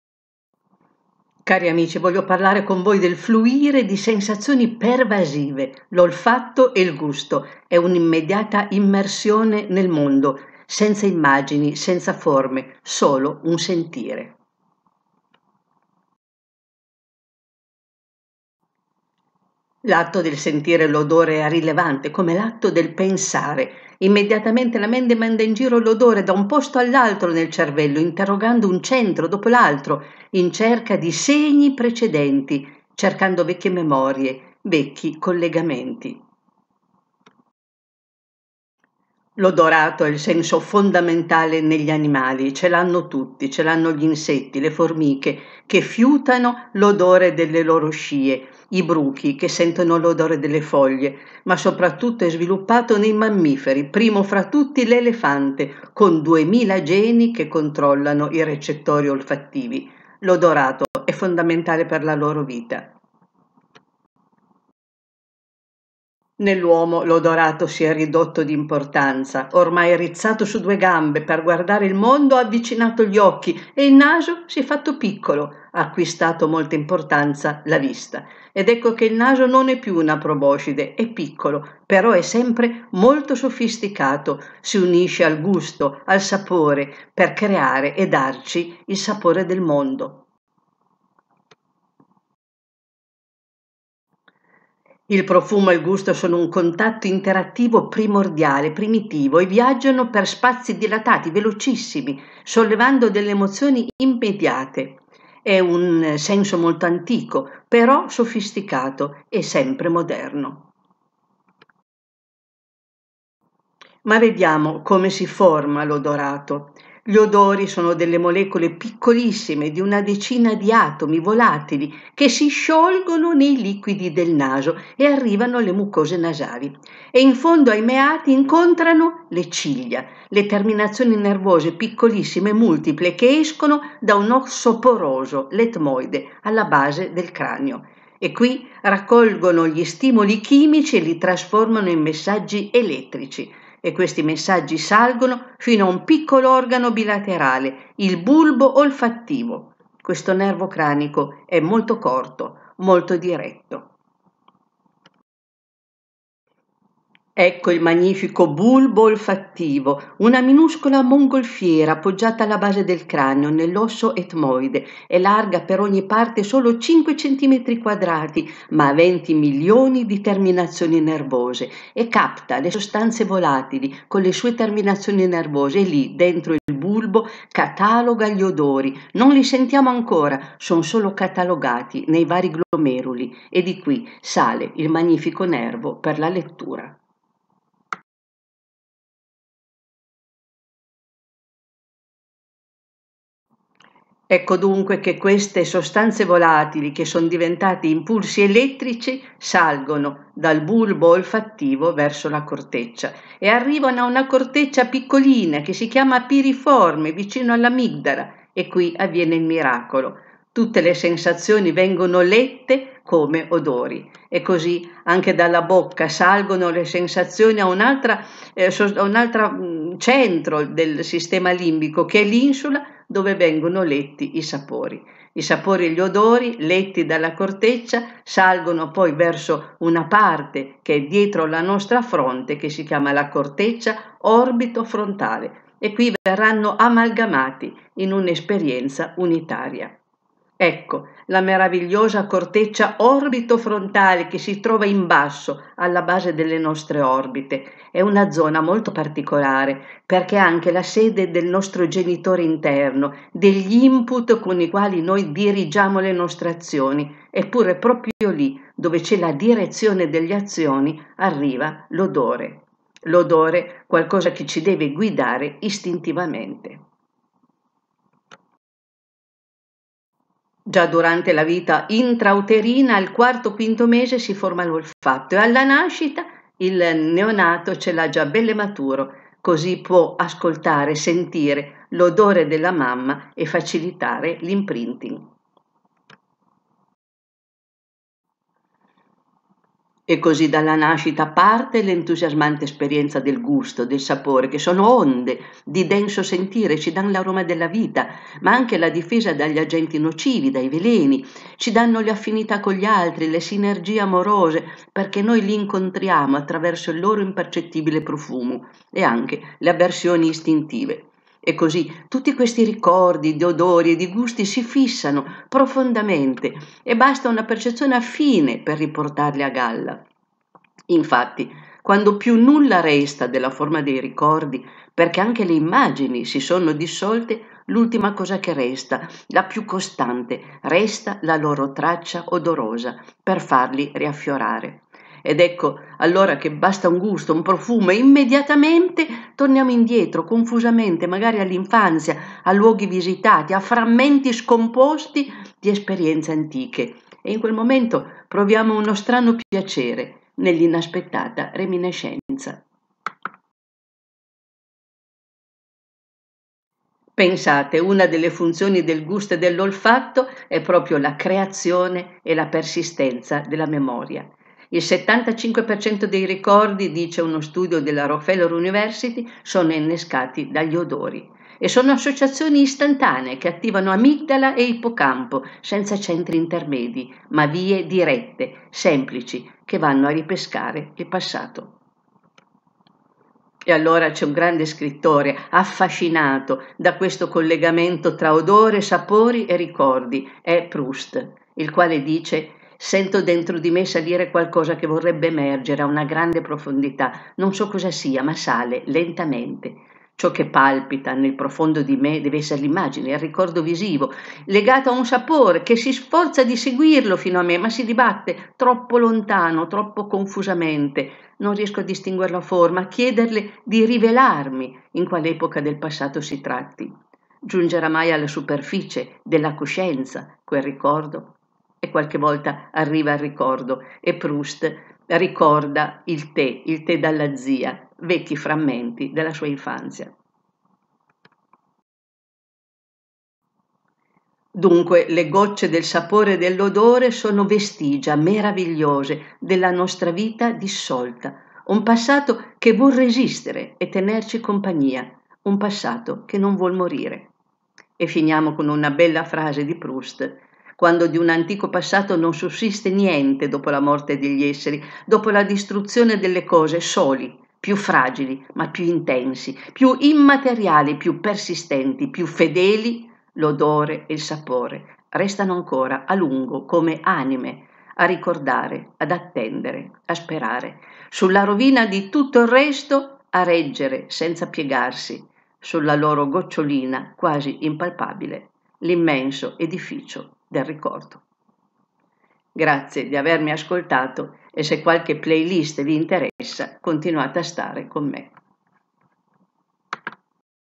Fluire di sensazioni pervasive. Olfatto e Gusto. Immediata immersione nel mondo!
L'olfatto e il Gusto sono sensi primordiali, i primi che si affacciano nel bambino per il suo imprinting con la madre, e continuano a regalarci sensazioni vitali, per darci piacere, difesa, attaccamento, amore. Facciamo un breve intenso viaggio insieme...